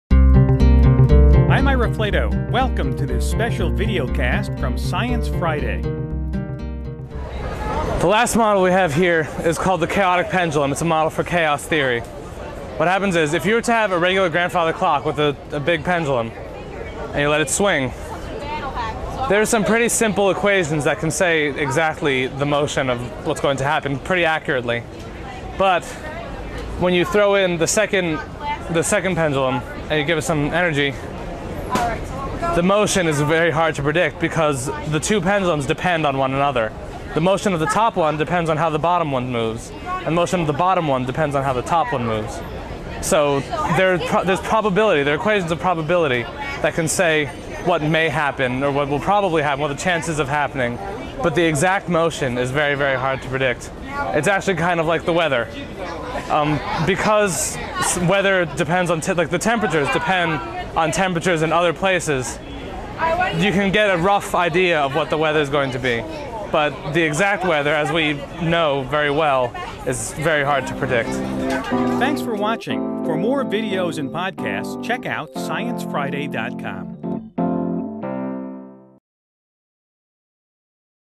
Wacky Swinging Pendulum
When two pendulums are attached to each other, their motion is impossible to predict.